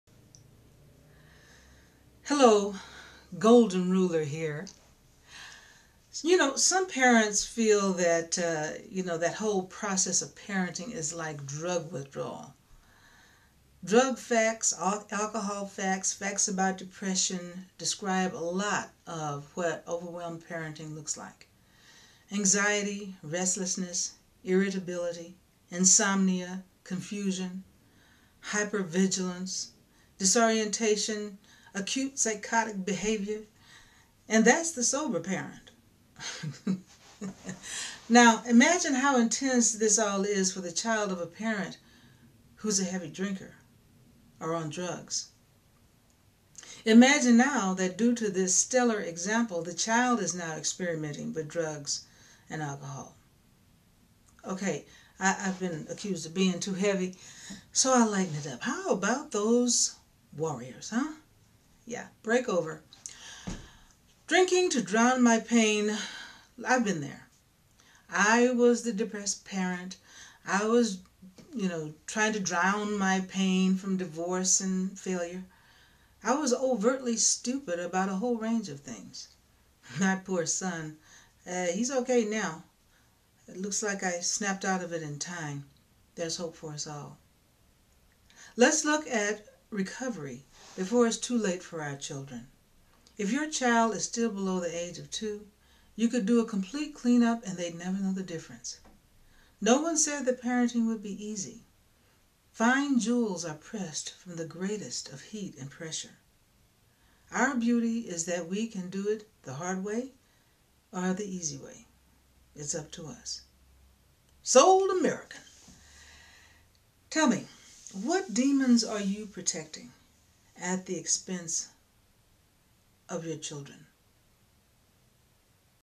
Some Parenting Feels Like Drug Withdrawal ... by Rowena Starling
Drugs, alcohol and depression facts describe a lot of what overwhelmed or lackluster parenting looks like; anxiety. restlessness, irritability, insomnia, confusion, hyper-vigilance, disorientation and sometimes acute psychotic behavior and that's if you're a sober parent!
What demons are you protecting at the expense of your children?